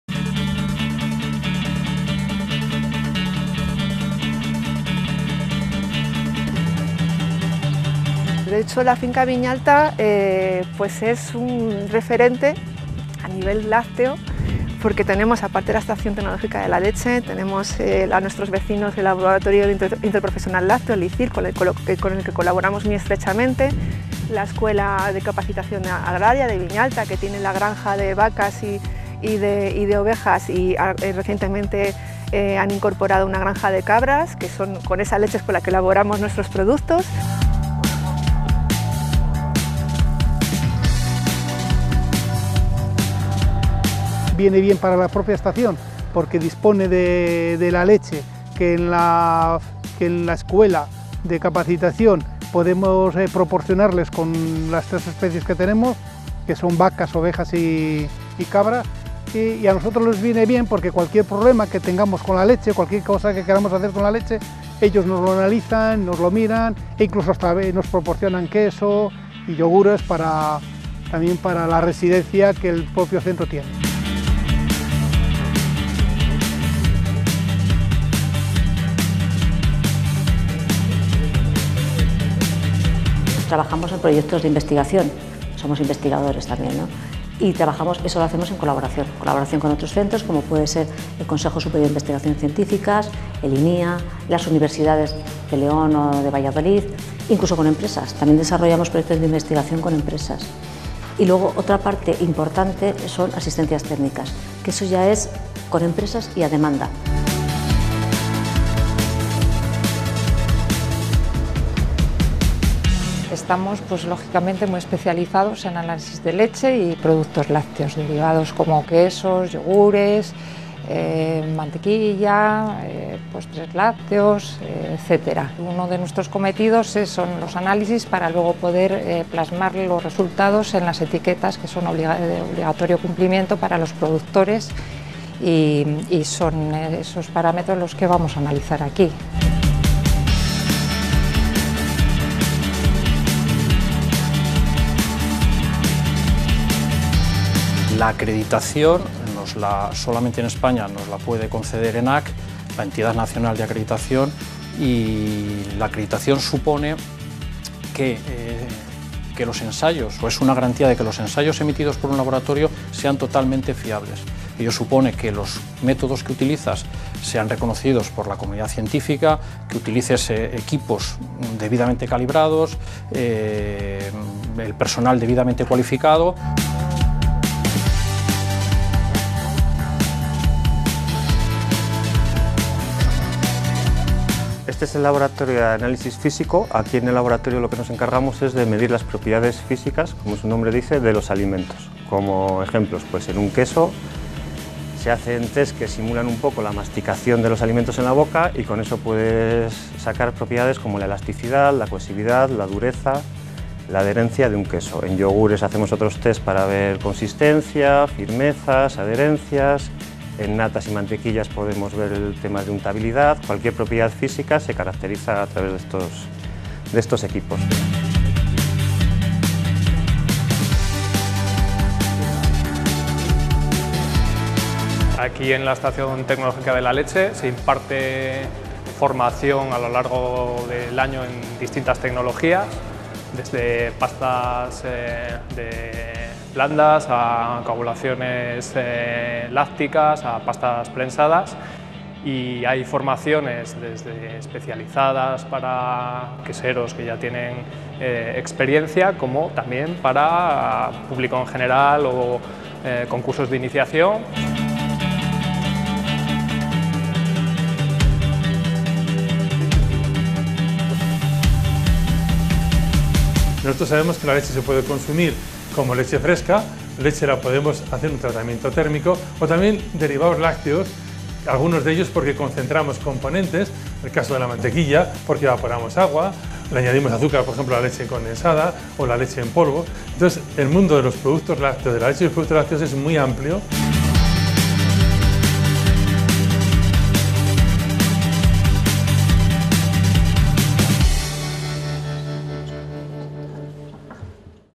PREVIO UnPaseoPorPalencia ESTACIÓN TECNOLÓGICA DE LA LECHE
Visitamos la Estación de la Leche, un centro de referencia en este sector para la región.